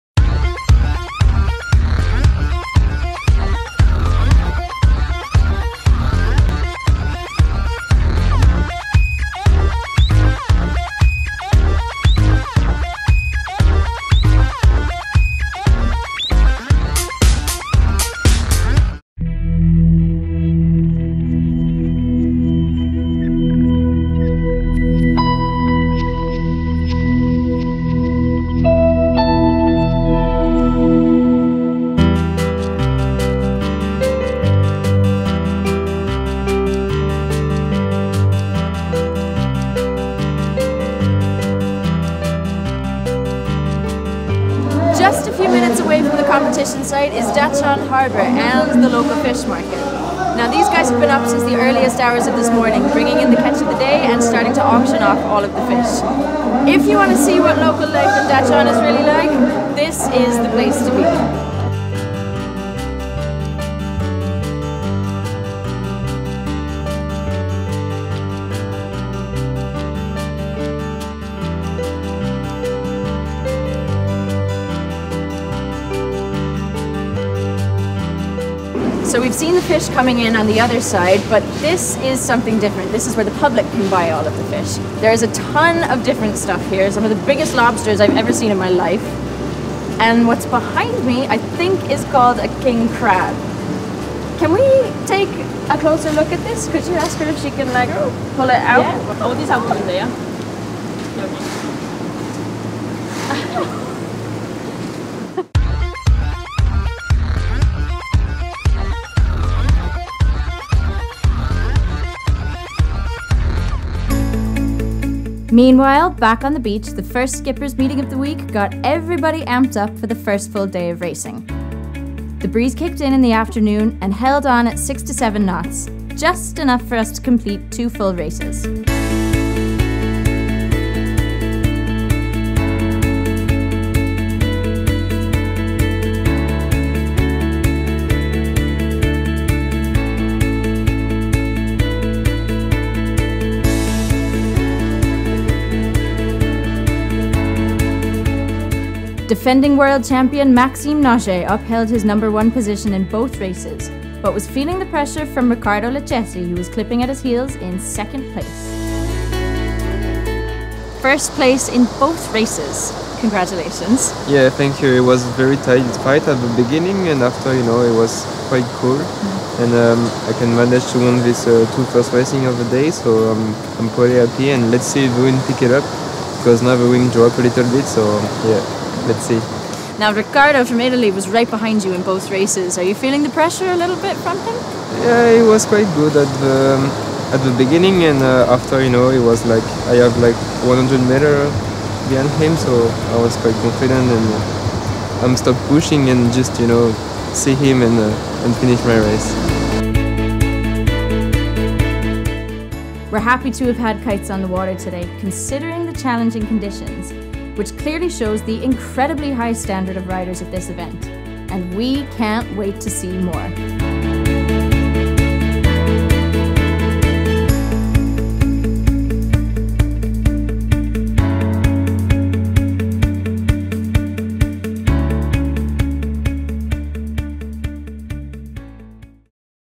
2017 IKA KiteFoil GoldCup & TT:R Open Korea - Day 2
Reigning International Kiteboarding Association (IKA) KiteFoil class champion Maxime Nocher began the defence of his title in Korea in perfect style—notching up two bullets from two races.

In light breezes that struggled to reach 8kts, the Monegasque rider put in a solid shift on the first day of the KiteFoil GoldCup’s opening stop of 2017 taking place on the Yellow Sea’s flat waters just off Boryeong’s pristine Daecheon Beach.